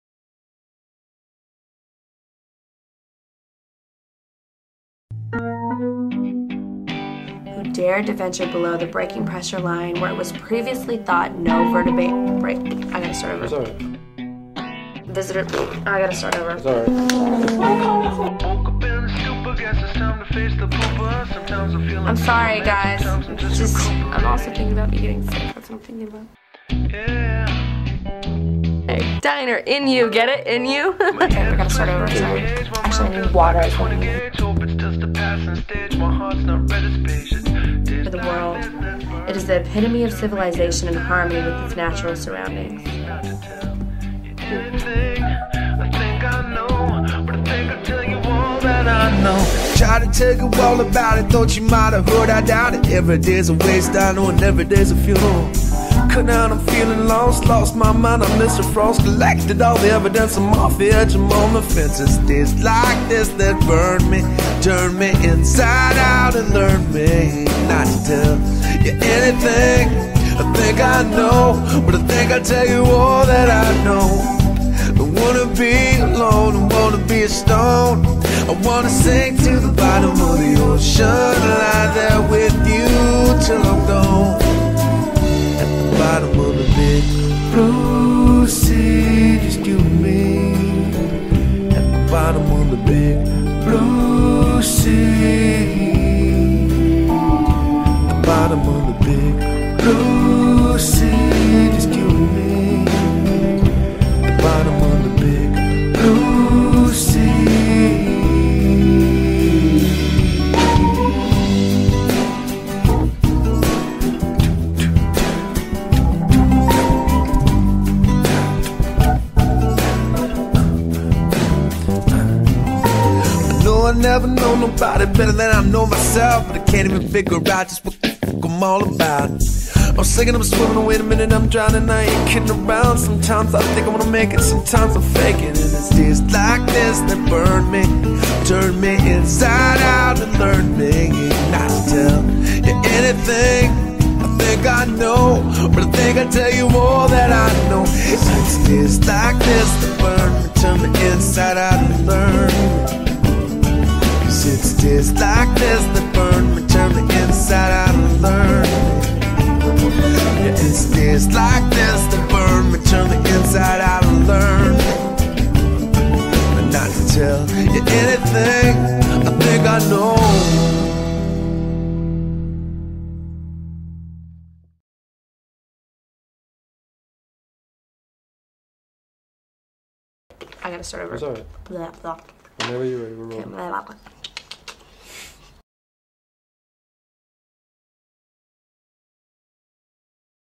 UCLA Walt Disney Imagineering (College Project) Recap (Big Blue Sea)
An Underwater Diner Concept (Recap Video) (Disney Imagineering UCLA Class)